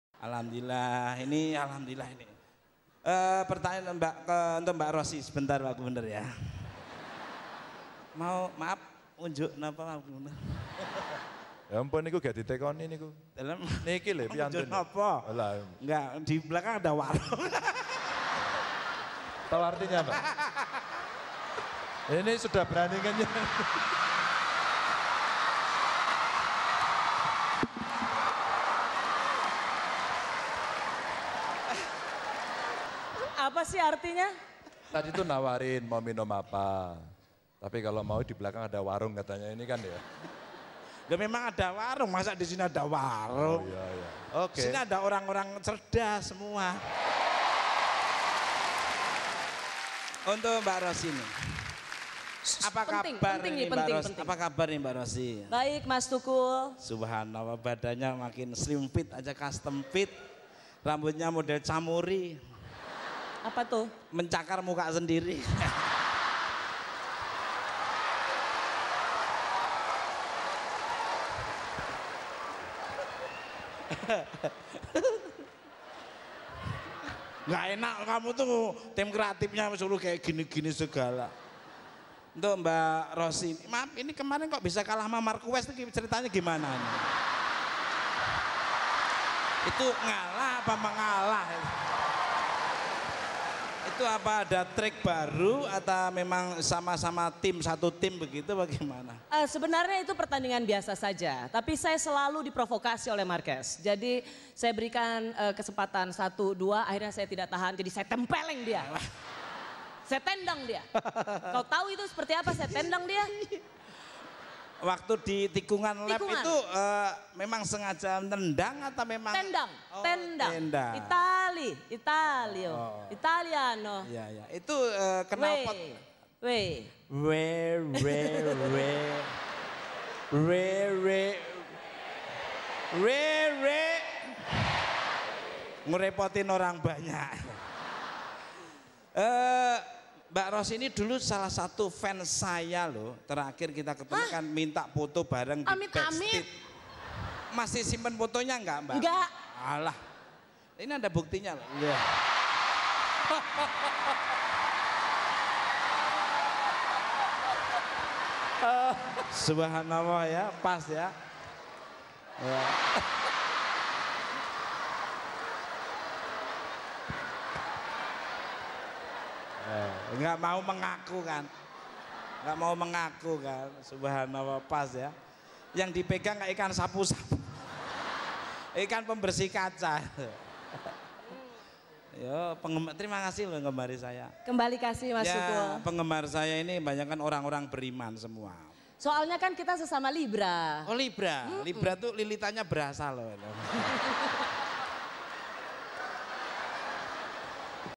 Rosianna Silalahi Nge-Fans dengan Tukul
Rosianna Silalahi dibuat seolah sebagai penggemar Tukul Arwana dengan ditampilkannya foto editan Rosi sedang memeluk mesra Tukul.